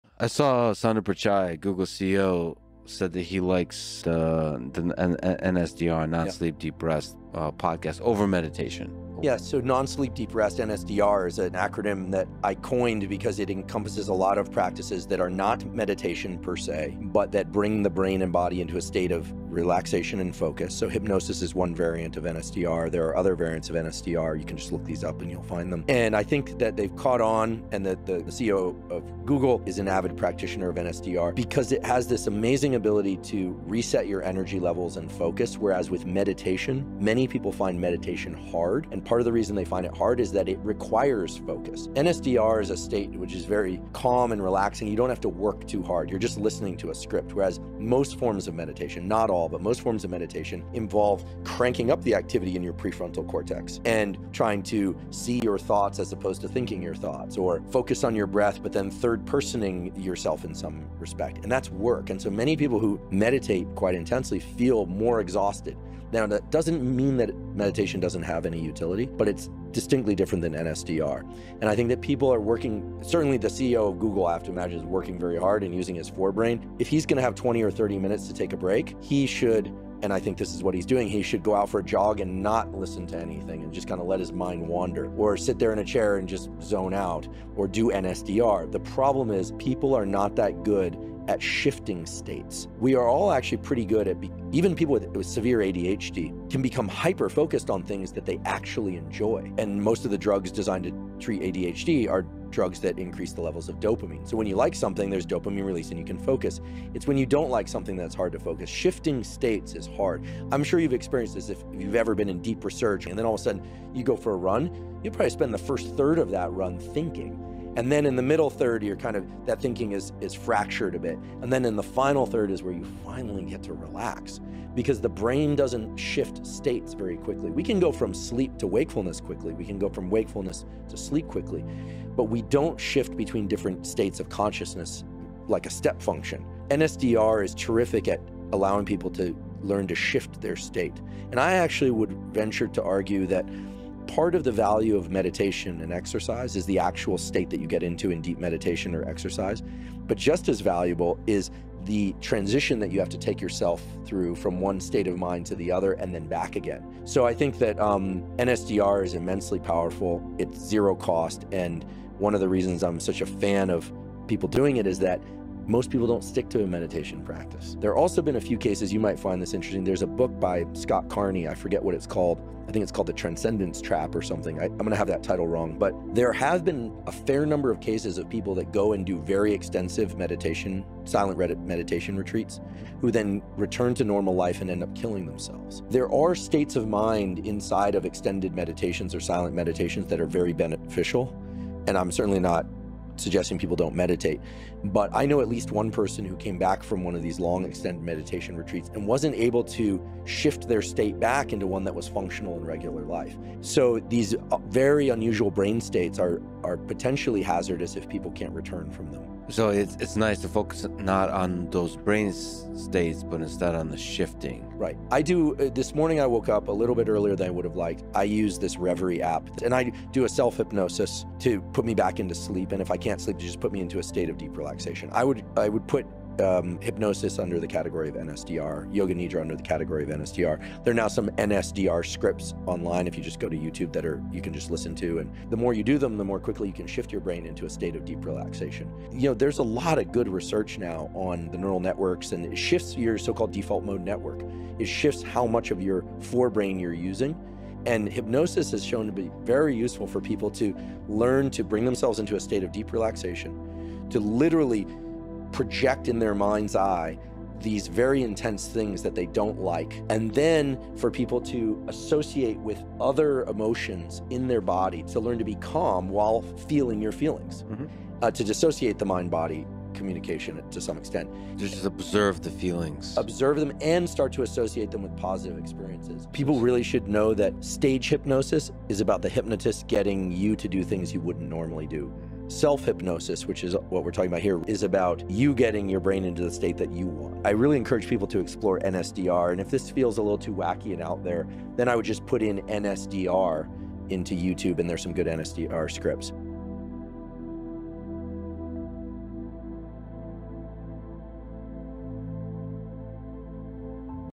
Neuroscientist: Watch this if you find it hard to meditate. Andrew huberman
Andrew Huberman explains non sleep deep rest and the benefits of self hypnosis.